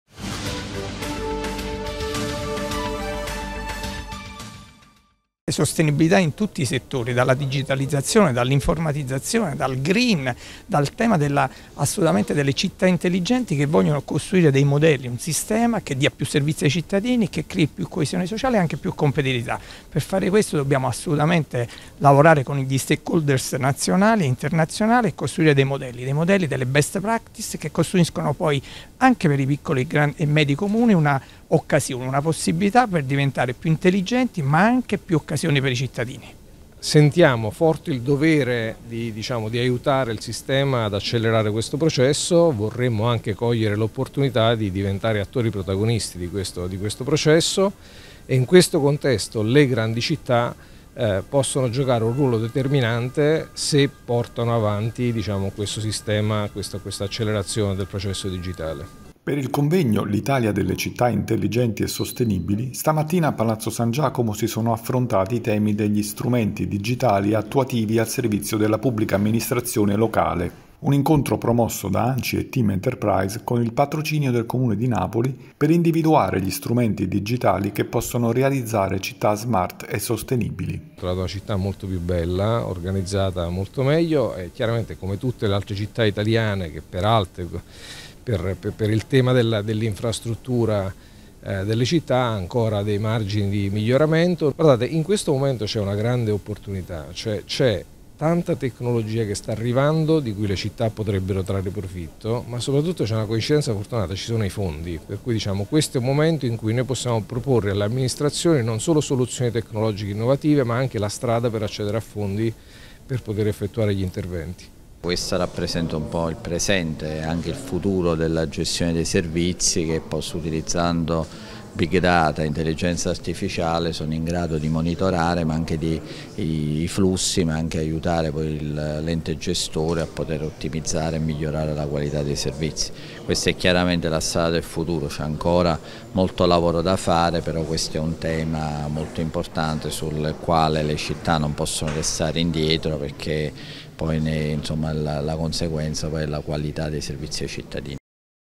Città smart e sostenibili, a Napoli il convegno con ANCI e Tim
A Palazzo San Giacomo il convegno “L’Italia delle città intelligenti e sostenibili", promosso da Anci e da Tim Enterprise con il patrocinio del Comune di Napoli: gli strumenti digitali al servizio della P.A. locale per realizzare città smart e sostenibili.